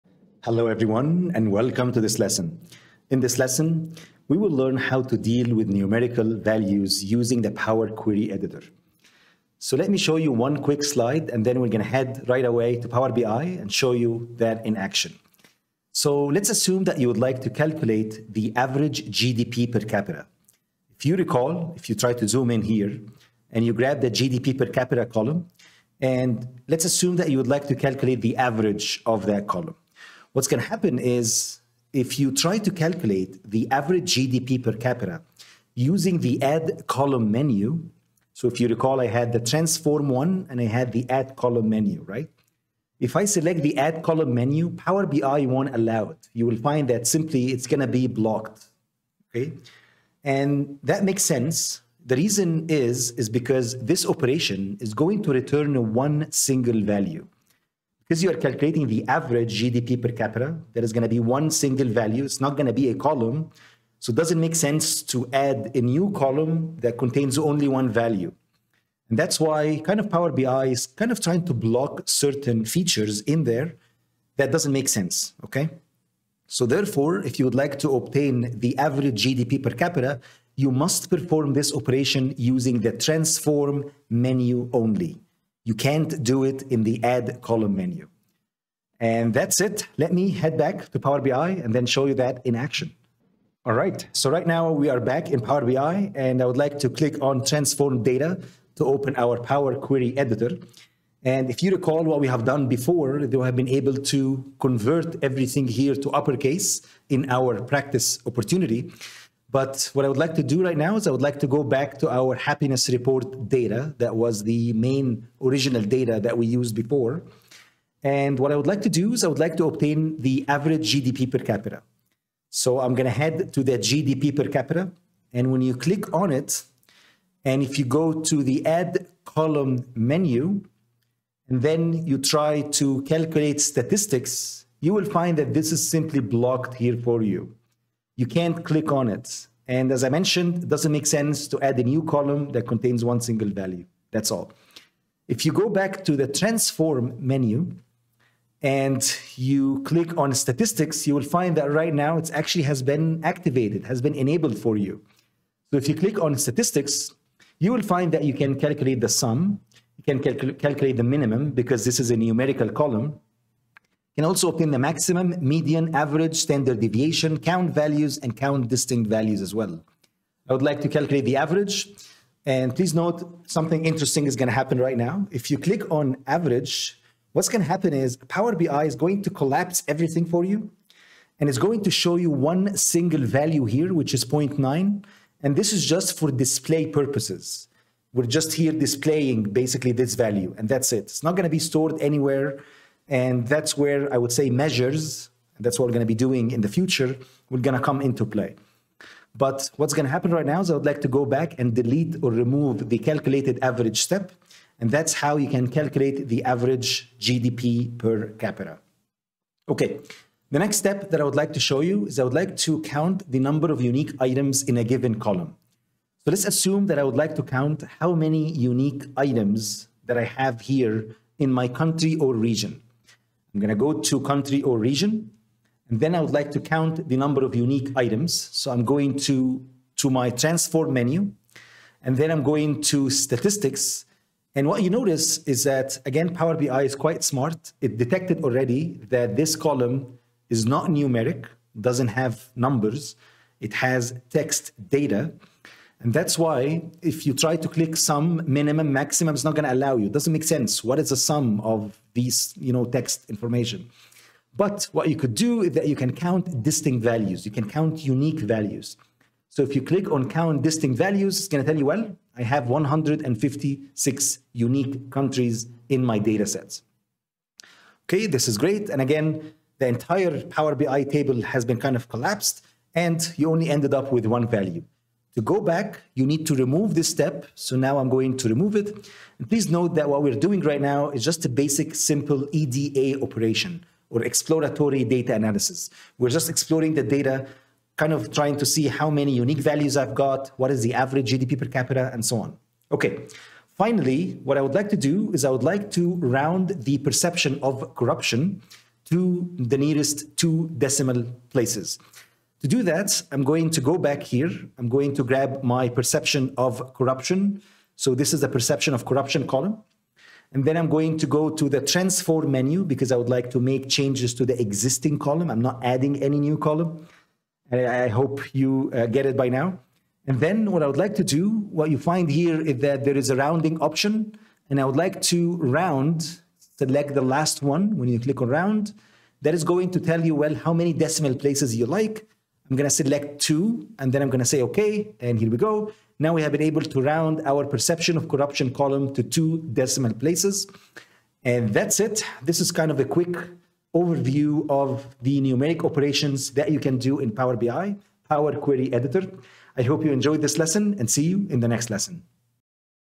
Power BI Tutorial: Handling Numerical Values
🚀 Welcome to your deep dive into handling numerical values within Power BI! 🚀

In today’s tech-driven world, data analysis is the linchpin of effective decision-making, and mastering numerical data is crucial in refining this process.

Whether you're a beginner or seasoned in using Power BI, handling numerical values efficiently is pivotal to creating insightful reports and dashboards.

Power BI Quick Mastery for Professionals + ChatGPT - New 2023!

Whether you're a Power BI beginner or an experienced data analyst, this tutorial offers insights that can help streamline your workflow and enhance your data models.

Thanks, and happy learning!

Ryan